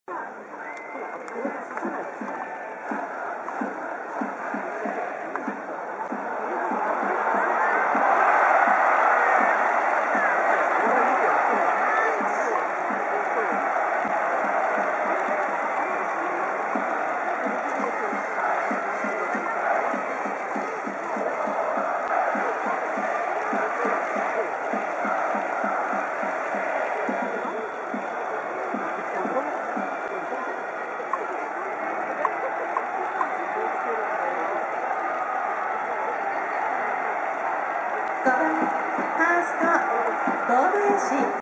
広島 野間峻祥 ツーベースヒット ヤクルト vs 広島 2024年9月19日(木) 神宮球場
2024年9月19日(木)　ヤクルト vs 広島　神宮球場　現地で撮影
7回表2アウト1塁で、野間峻祥がレフトのライン際にツーベースヒット。